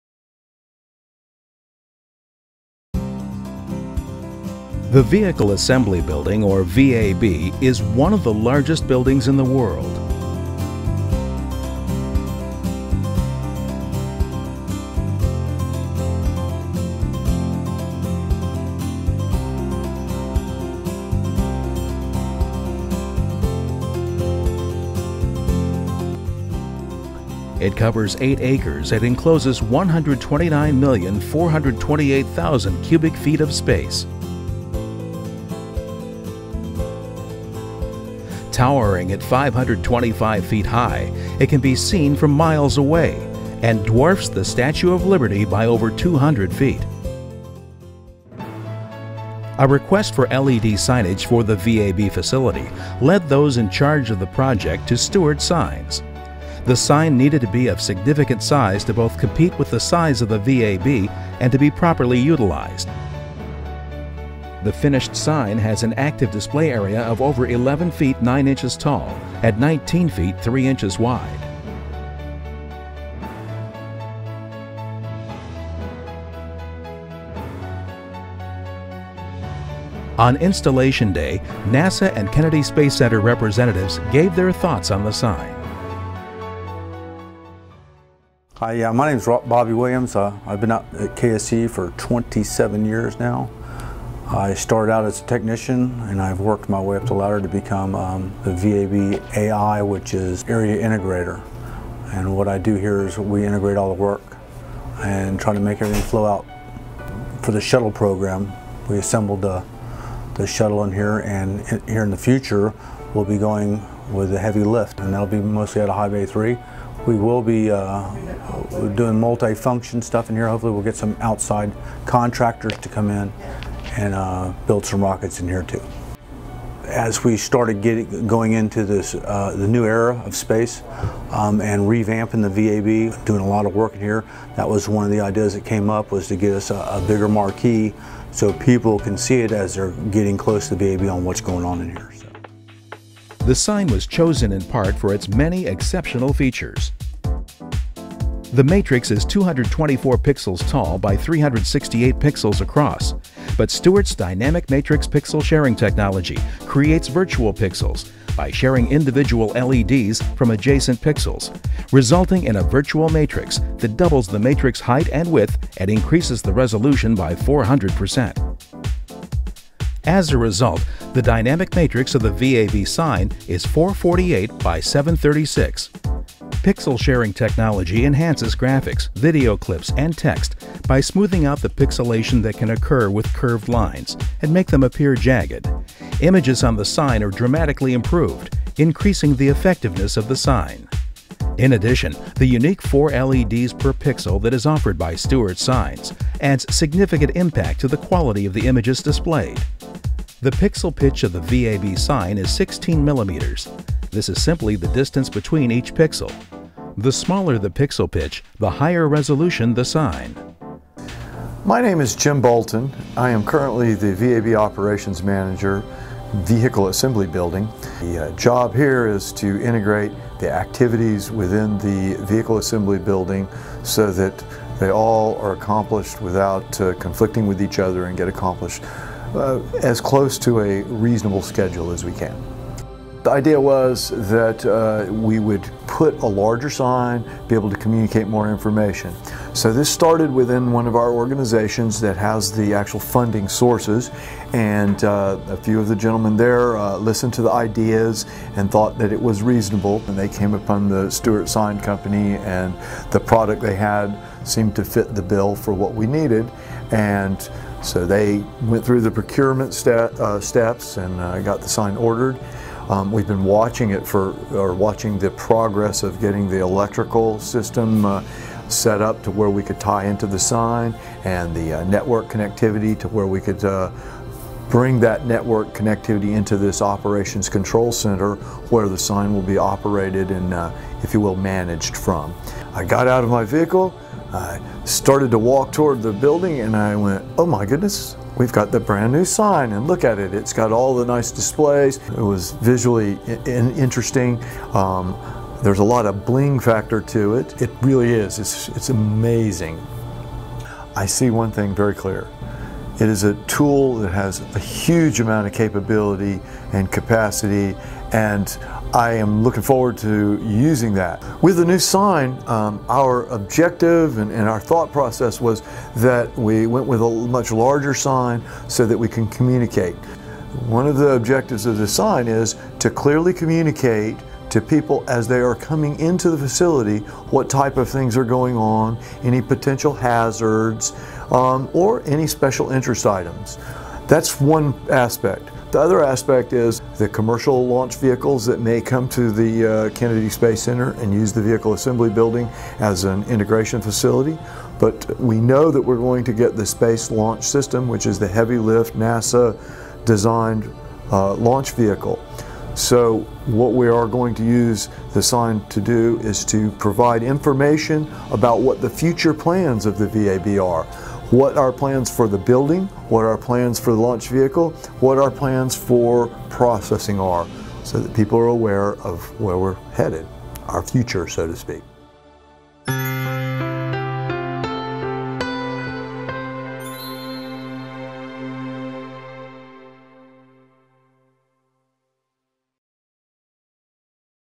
LED Sign Testimonial for NASA Vehicle Assembly Building
The sign company of rocket scientists?  We like to think so. Stewart Signs was selected by NASA for a new LED sign at the Vehicle Assembly Building.  The sign has an active display area of over 11' high and 19' wide with a matrix of 224 pixels tall by 368 pixels wide with a 16mm pitch.